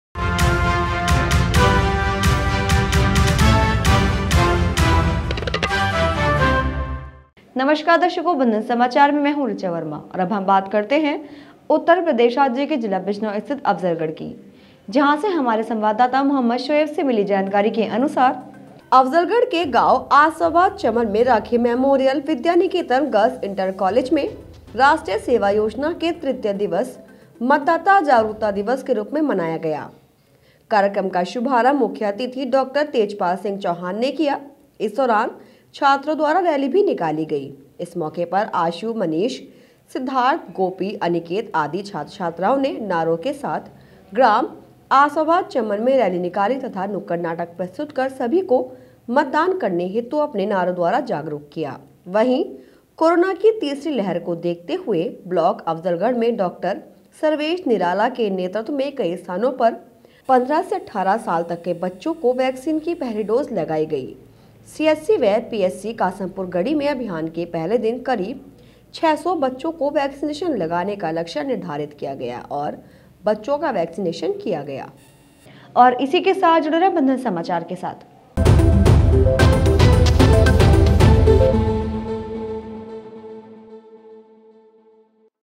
एनएसएस शिविर के तहत निकाली गयी जागरूकता रैली, वैक्सीनेशन अभियान भी चलाया गया
अफजलगढ़ के गांव आसफाबाद चमन में राखी मेमोरियल विद्या निकेतन गल्र्स इंटर कॉलेज में राष्ट्रीय सेवा योजना के तृतीय दिवस मतदाता जागरूकता दिवस के रूप में मनाया गया, कार्यक्रम का शुभारंभ मुख्य अतिथि डॉक्टर तेजपाल सिंह चैहान ने किया|